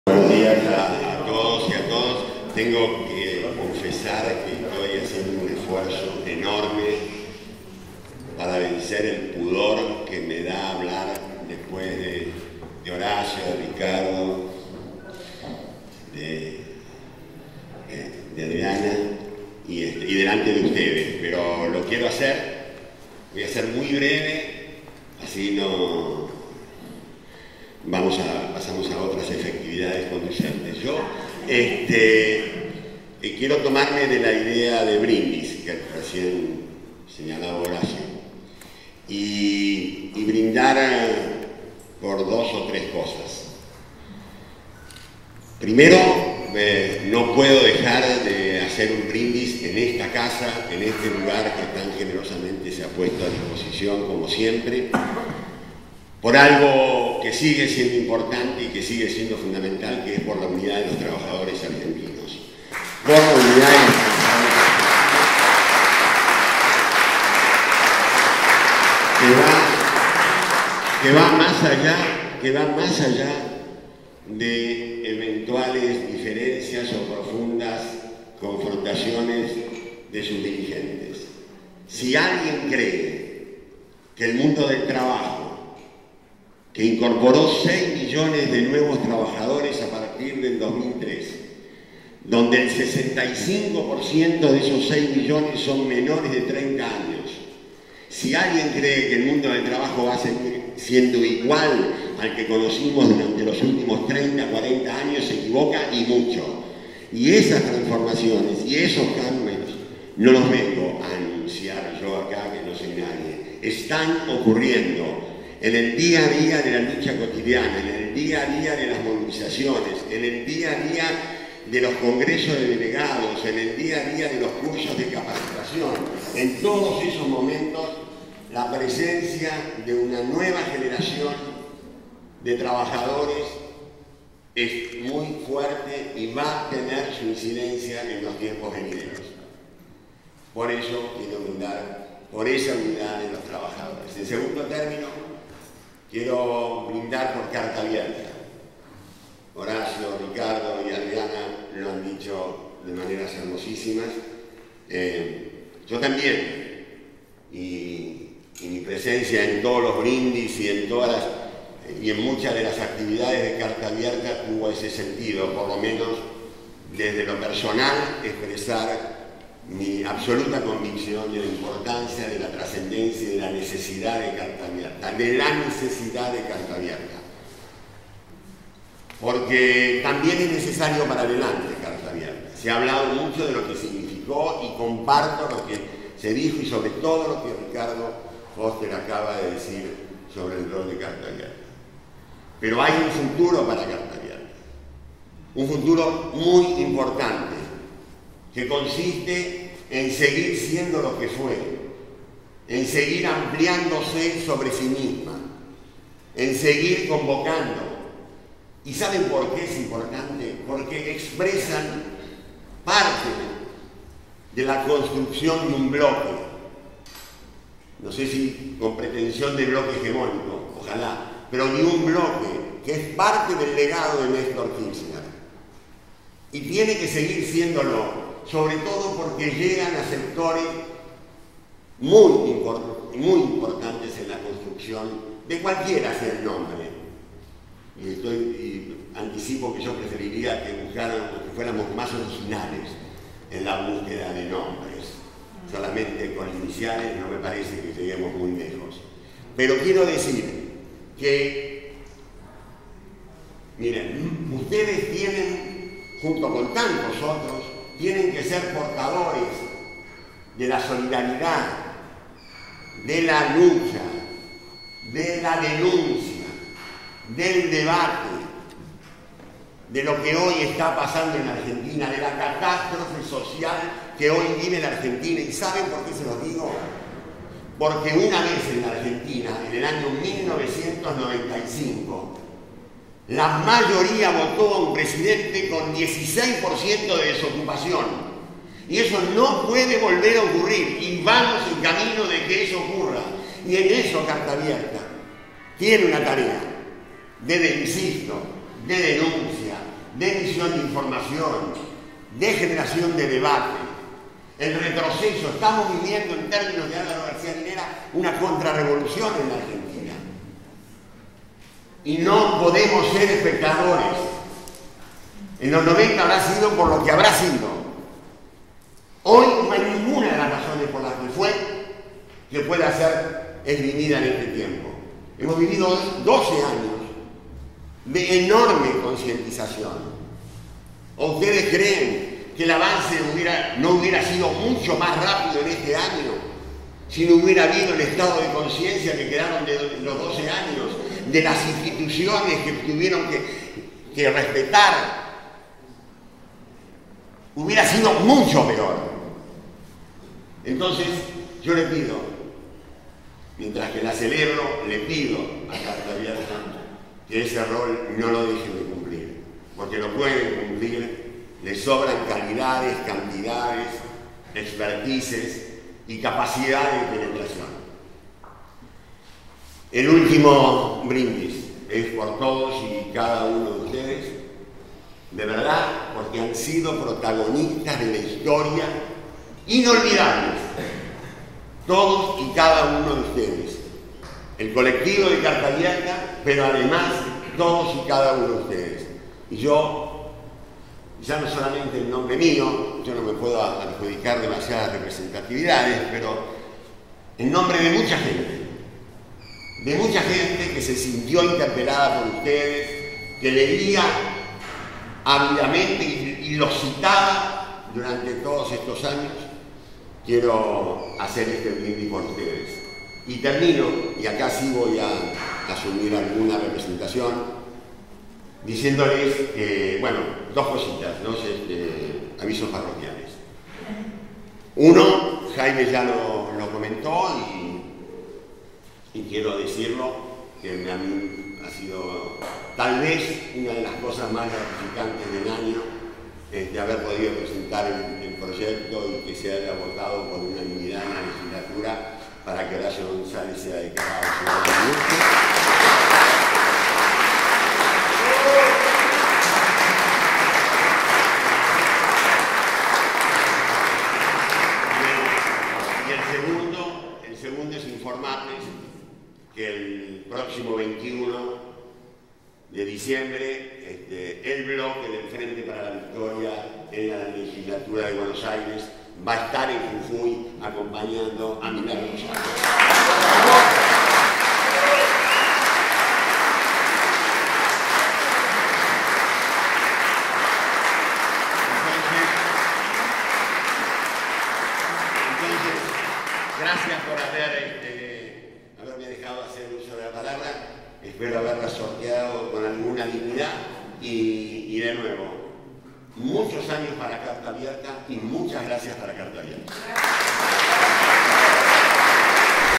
11CA Carlos Tomada
Brindis de fin de año durante la asamblea de Carta Abierta realizada en la sede de la CTA. 17 de diciembre de 2016.